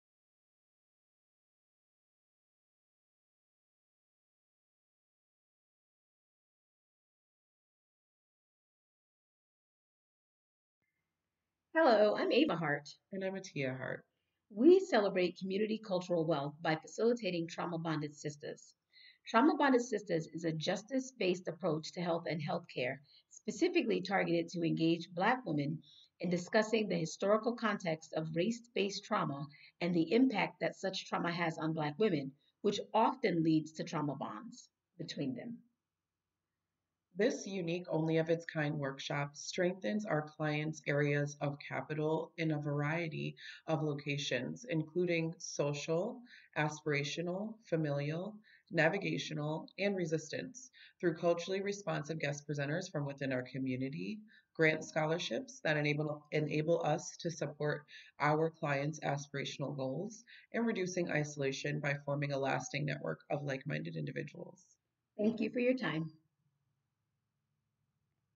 EMDRIA Black History Month 2024 I Ava Hart and Atia Hart
EMDRIA Member Ava Hart, LCSW and Atia Hart answer the question “Describe some resources that you strengthen/install with your Black or African American identifying clients. How do you encourage them to embrace their Community Cultural Wealth?”